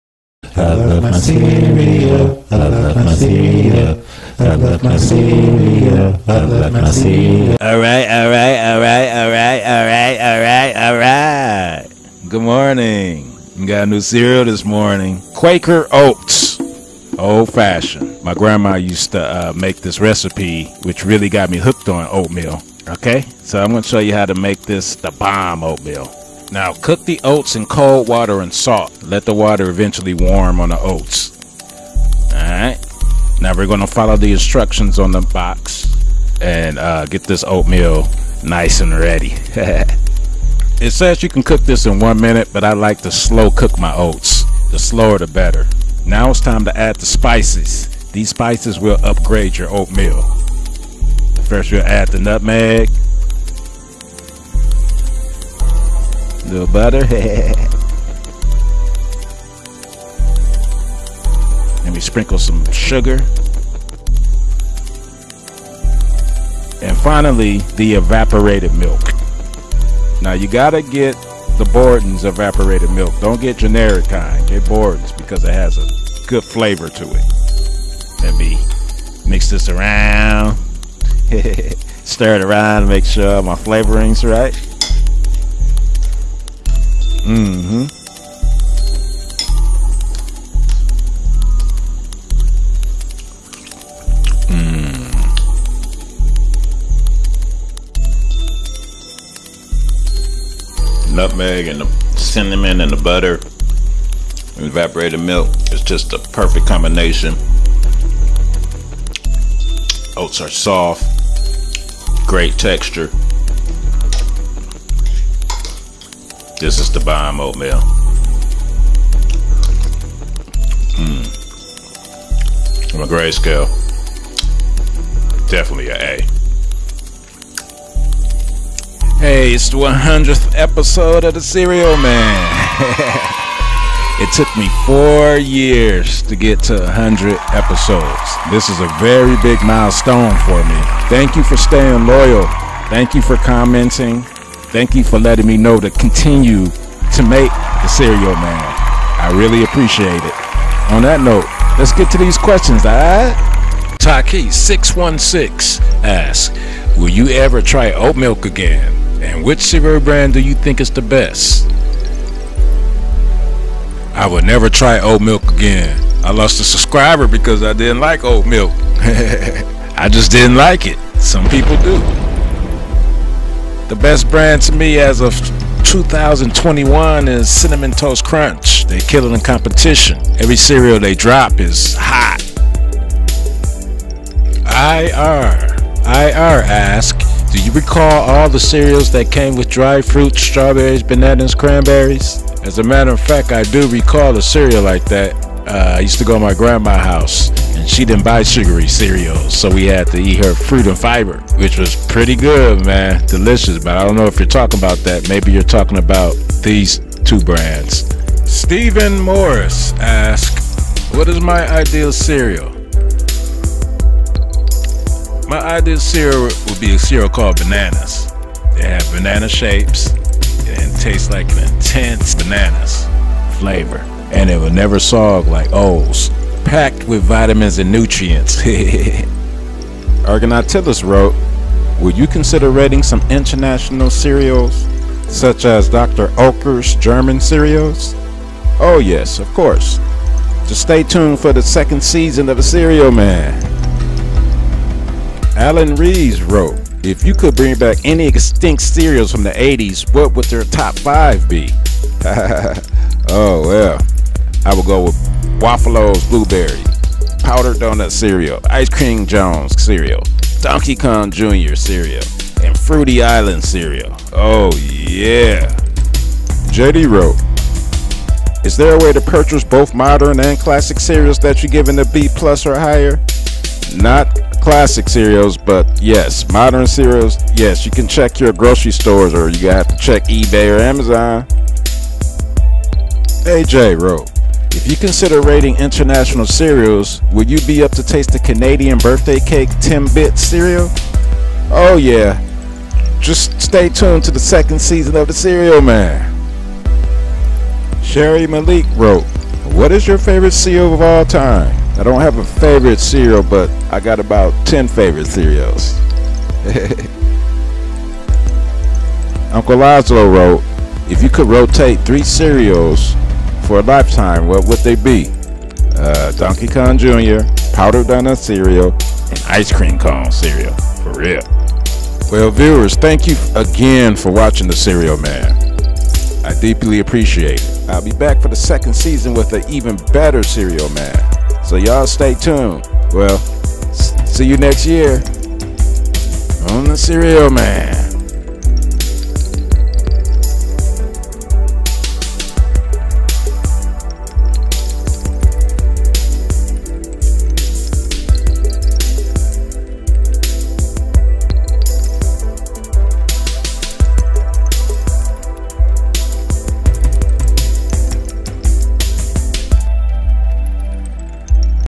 *The Cereal Man* 100th Episode Special
Alright alright alright! I finally reached 100 videos on the Cereal Man Show!
In this episode I make Original Quaker Oats ( my Grandma recipe) and I do a question and answer from viewers.

Whatever you do, you just can't quit (andre3000)